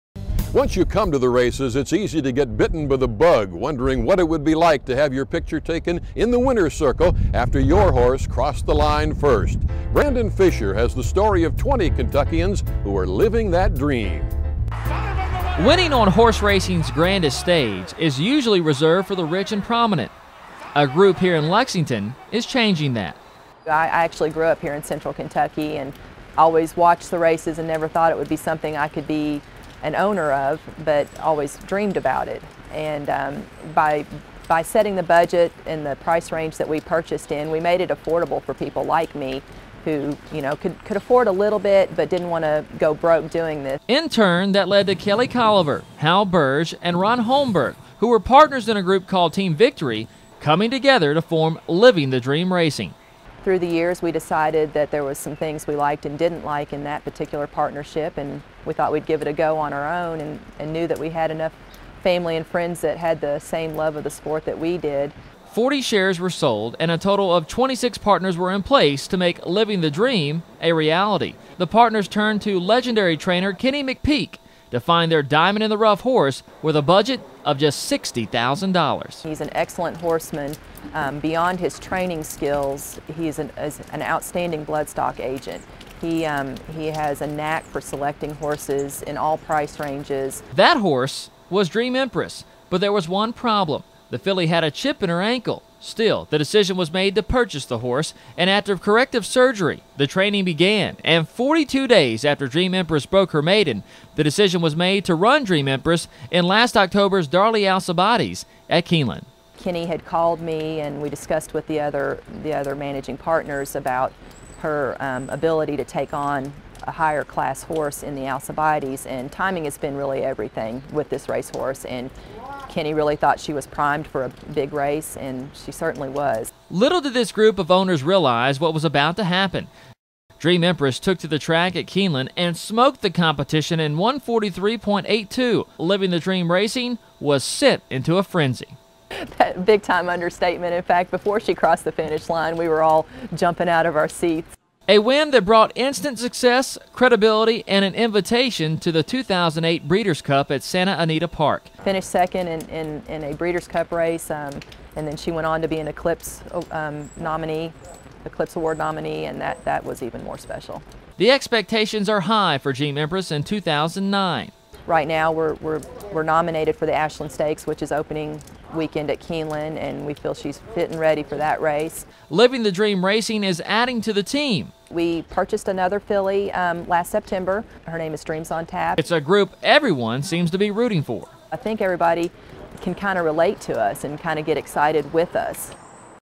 Livin the Dream Racing
Livin the Dream Racing is a partnership comprised of more than 20 friends who are experiencing success right out of the gate. They also hold an unofficial Keeneland record for most people ever in a winner's circle photo.